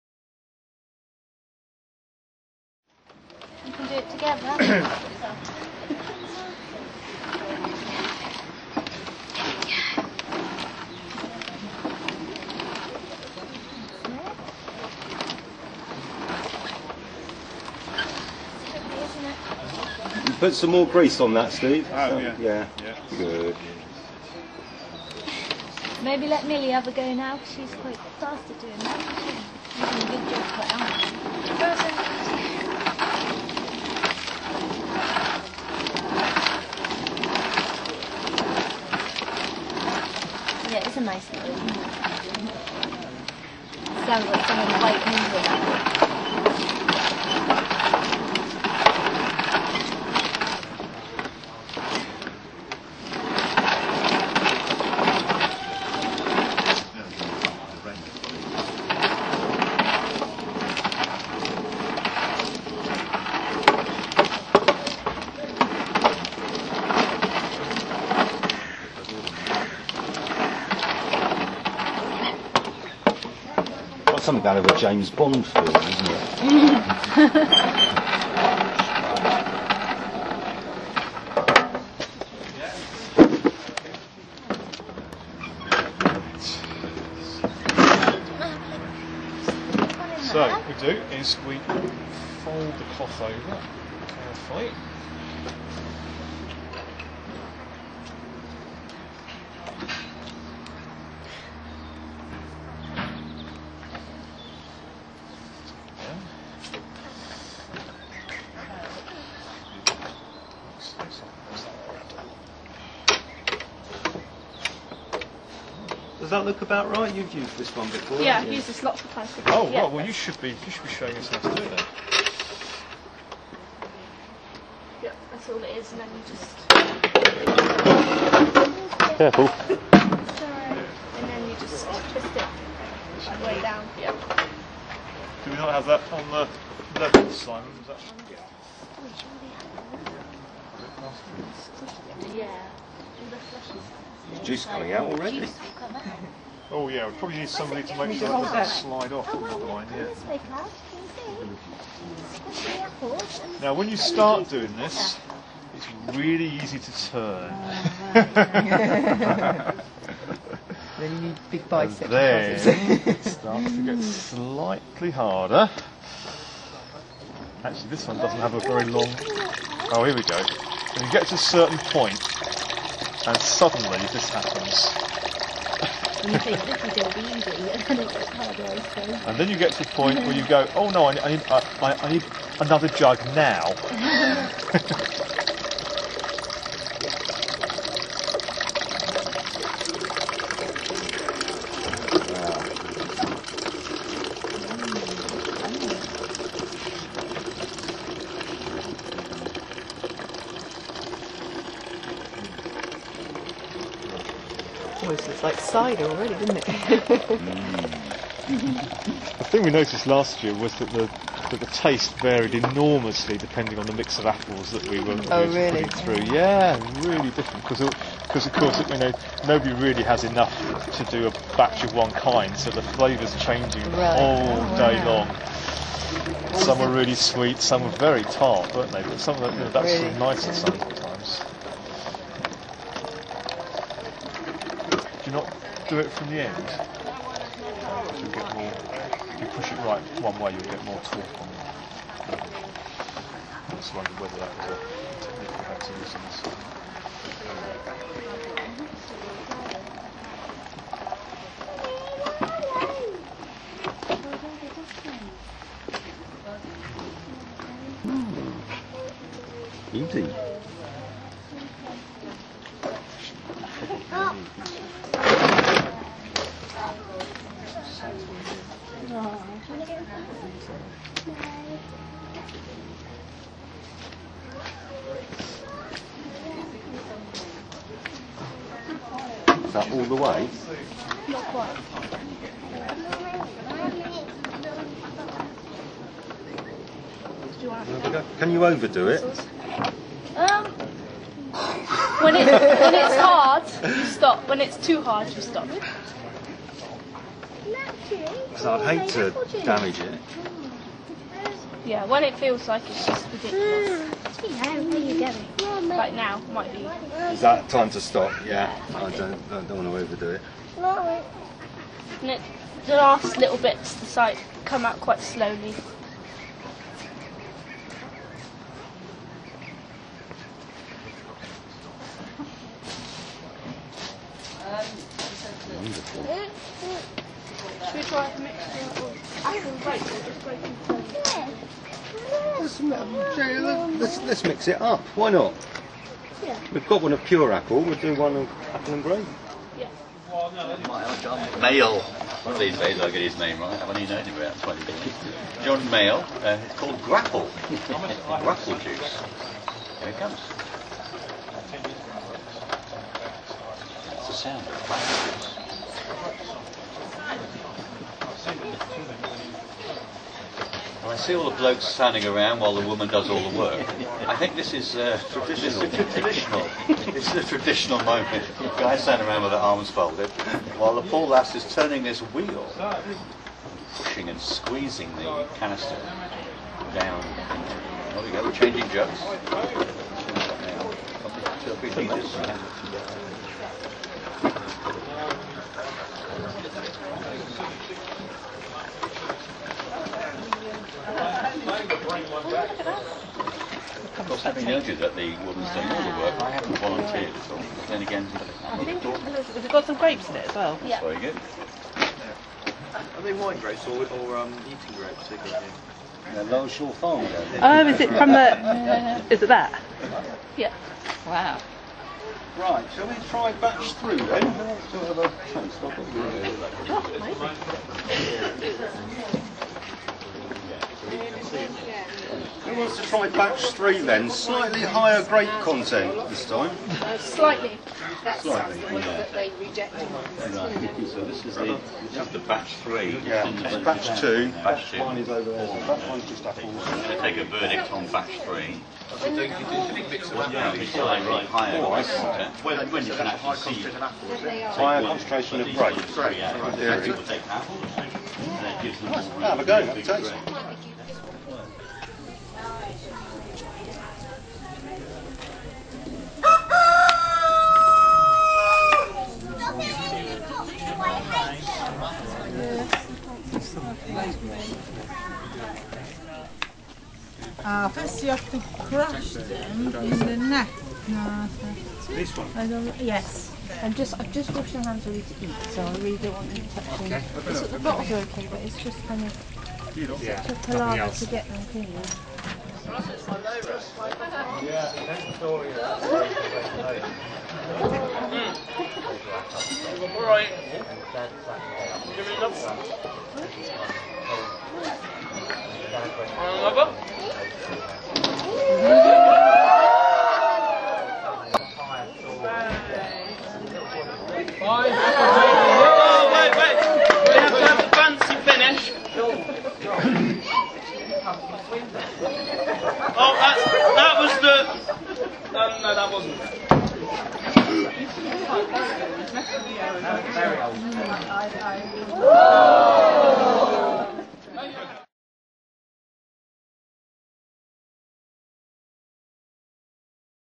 Apple day 2012
Lower Shaw Farm hosted 2 days of apple activities on 6th and 7th October 2012, including apple pressing, apple juggling and, I kid you not, apple storytelling.